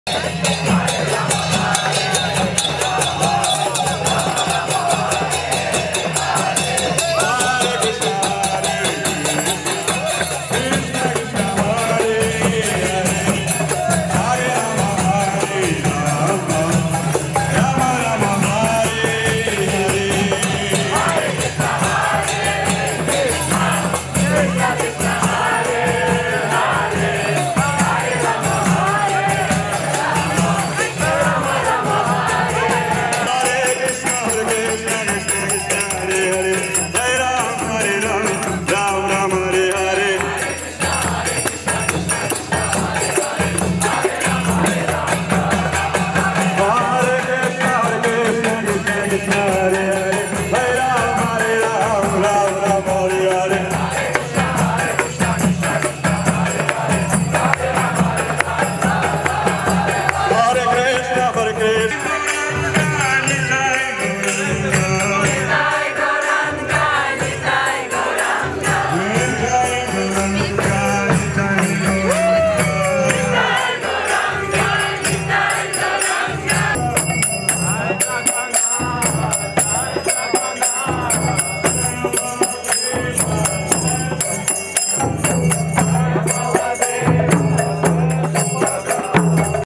Ratha-yatra
У Харківському парку відпочинку влаштували Індійське свято Колесниць (Ratha yatra).